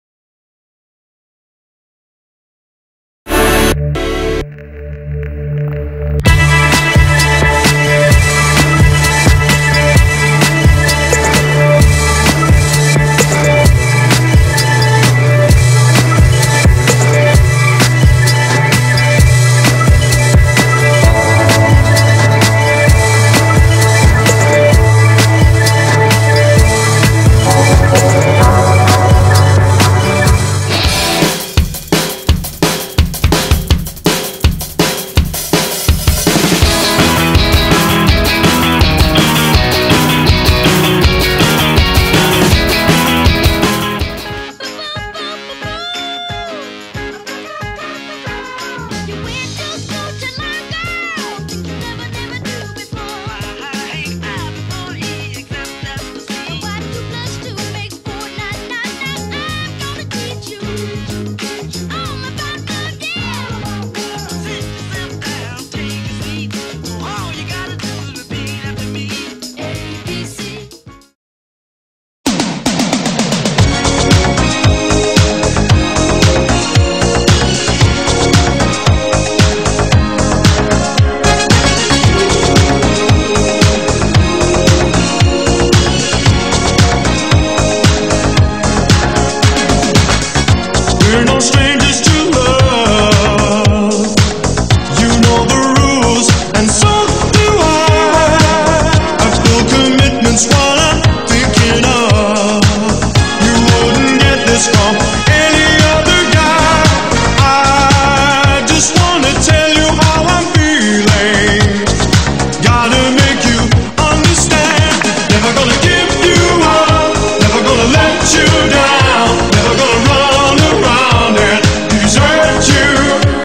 A Simple Real-time audio DMX light - Music Hack Day
Using the Arduino, Peter Knight's library "DMXSimple" and processing.